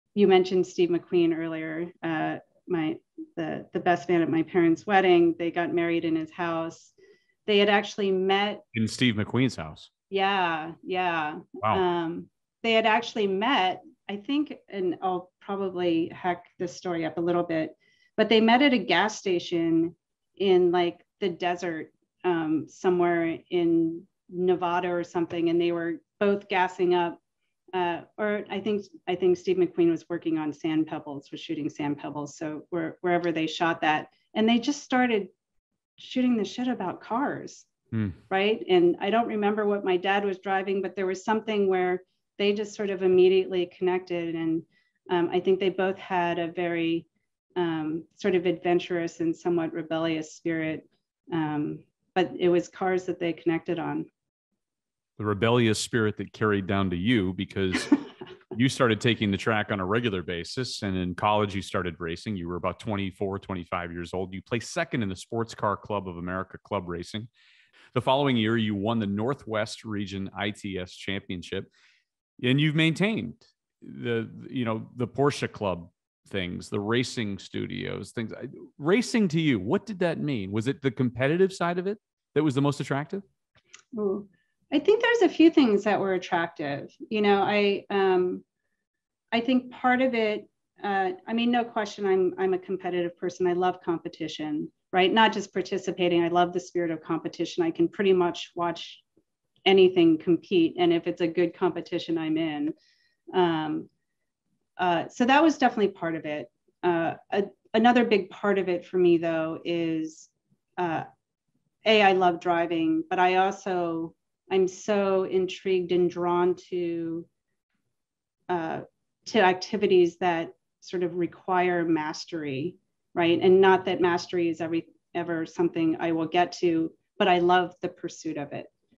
Getting Married at Steve McQueen's House
Clip from Cars and Culture #80 - Best of 2022 Part 1

After an incredible 2022 it's time to take a look back on some of the incredible guests we've had on this year. First up is Coldplay's bassist Guy Berryman, video game developer Kiki Wolfkill, and head of design at Maserati Klaus Busse.